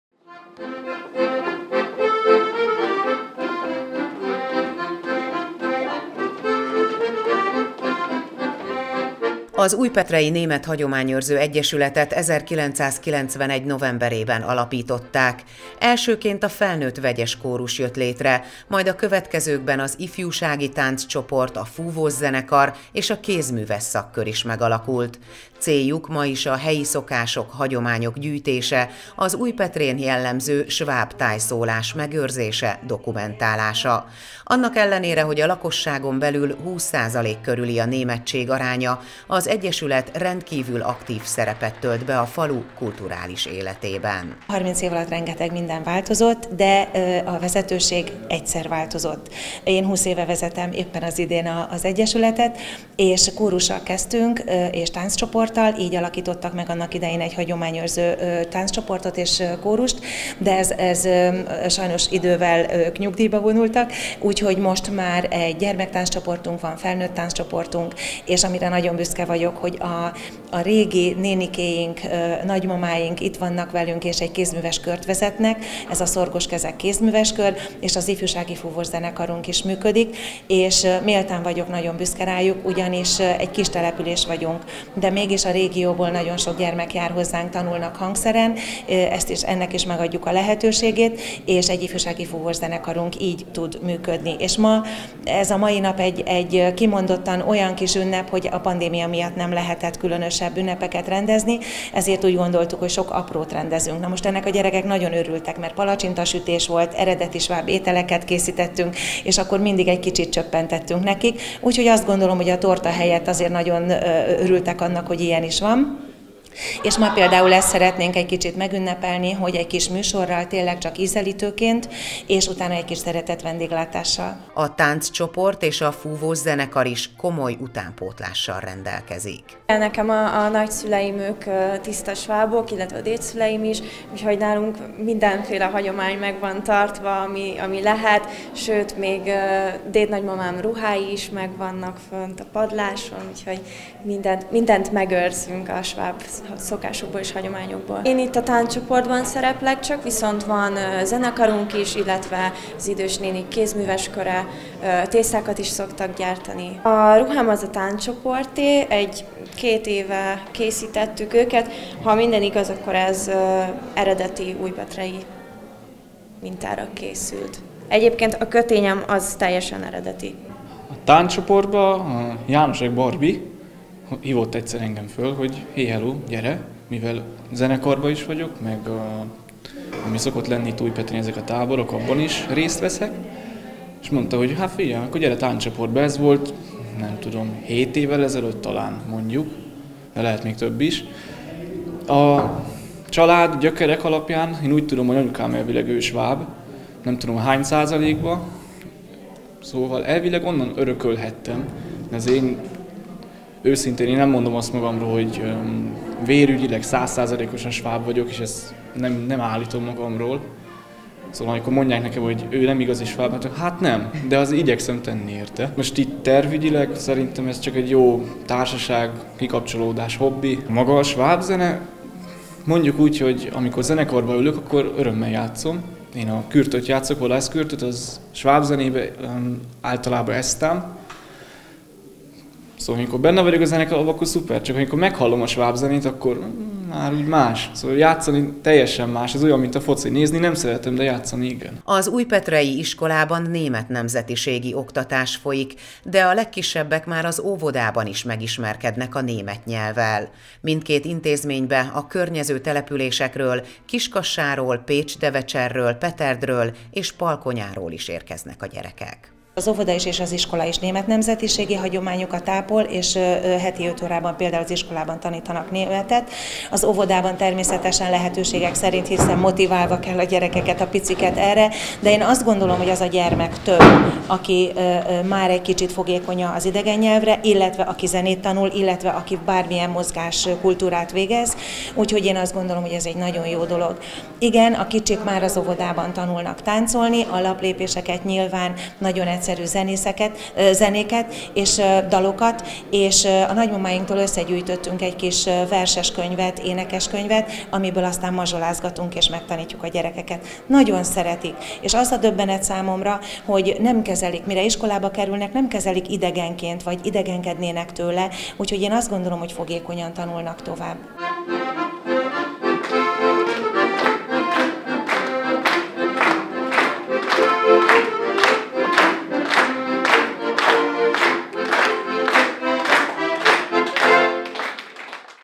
Harmincéves az Újpetrei Német Hagyományőrző Egyesület
Folytatta jubileumi programsorozatát az Újpetrei Német Hagyományőrző Egyesület. A magyar és német népesség békében él egymás mellett, és ezt a szép eszmei tradíciót az egyesület munkája is példázza, képviseli. A harmincéves egyesület már az óvodában elkezdi a sváb kultúra megismertetését a gyerekkel.

 Der Deutsche Traditionspflegeverein Ratzpeter hat seine Programmreihe zum Jubiläum fortgesetzt. Die ungarische und deutsche Bevölkerung lebt friedlich nebeneinenader und diese schöne ideelle Tradition ist auch in der Arbeit des Vereins vertreten. Der 30 Jahre alte Verein beginnt schon im Kindergarten, die Kinder mit der schwäbischen Kultur bekannt zu machen.